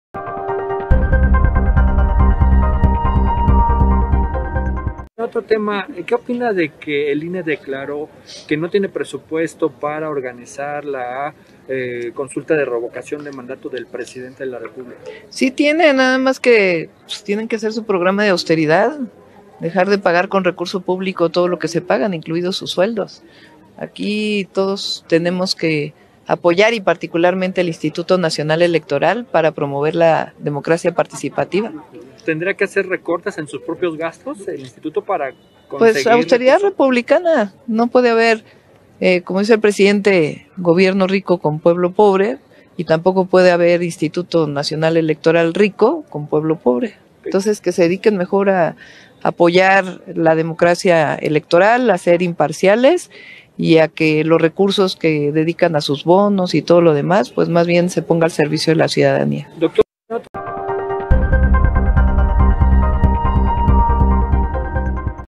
SHEINBAUM LE MANDA DURO MENSAJE A CONSEJEROS DEL INE POR PEDIR MÀS DINERO.
SHEINBAUM LE MANDA DURO MENSAJE A CONSEJEROS DEL INE POR PEDIR MÀS DINERO PARA LA CONSULTA DE LA REVOCACIÒN DE MANDATO.